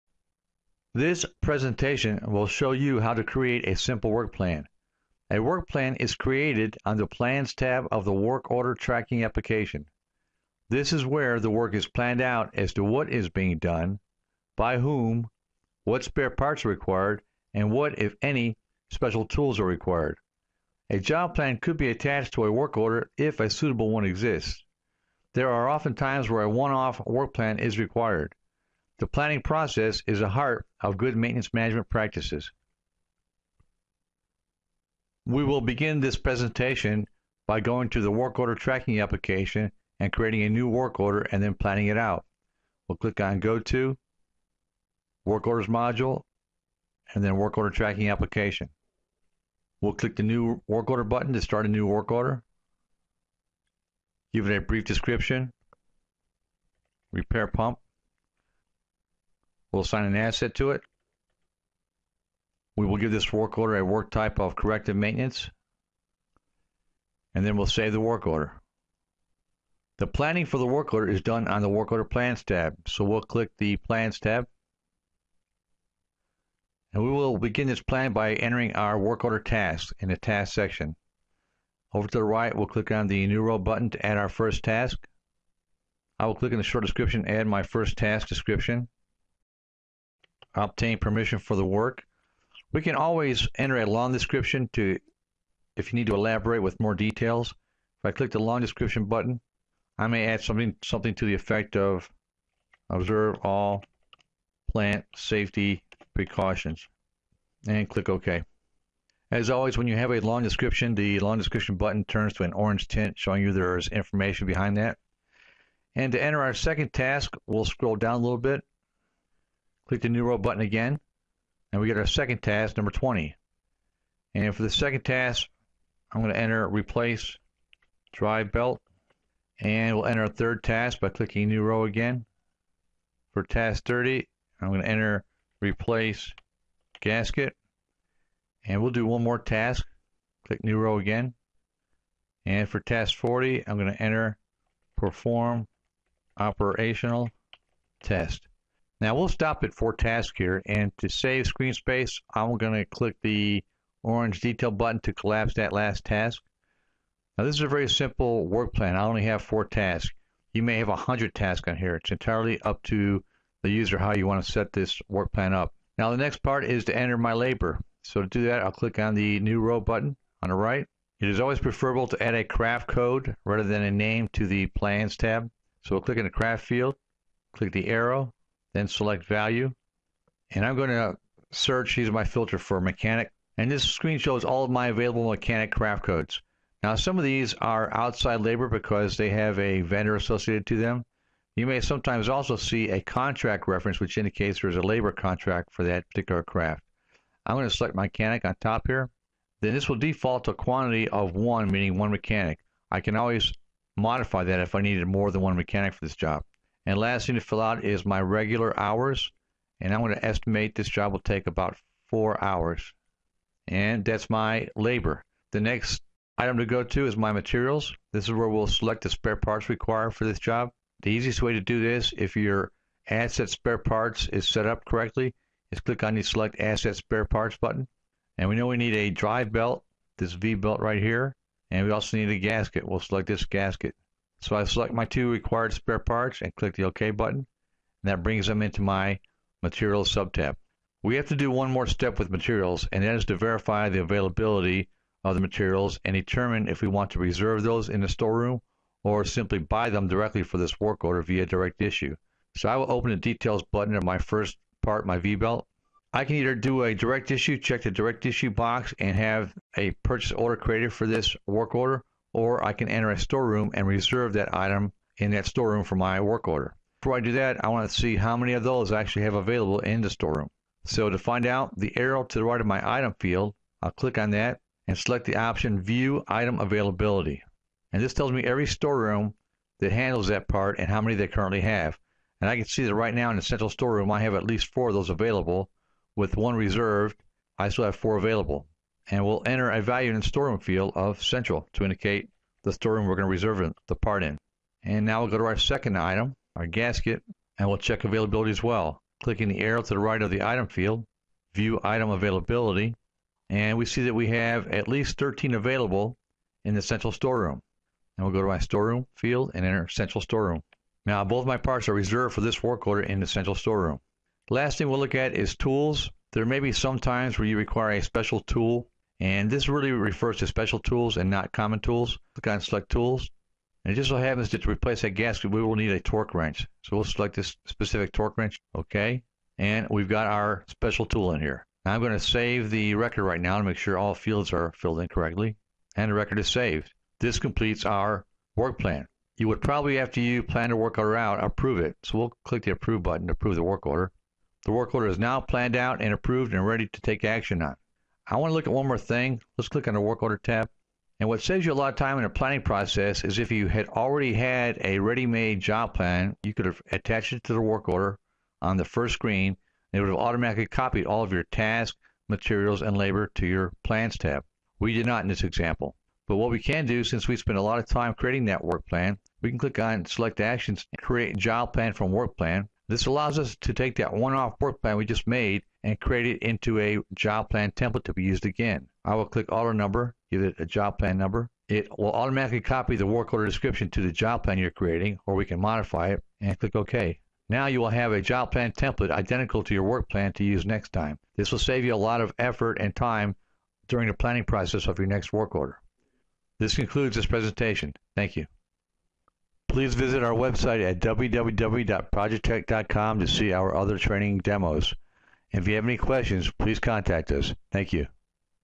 Maximo 7.5 Basic Work Planning
Create a basic work order plan with Maximo. Apply labor, parts/materials, and task requirements to work order plan.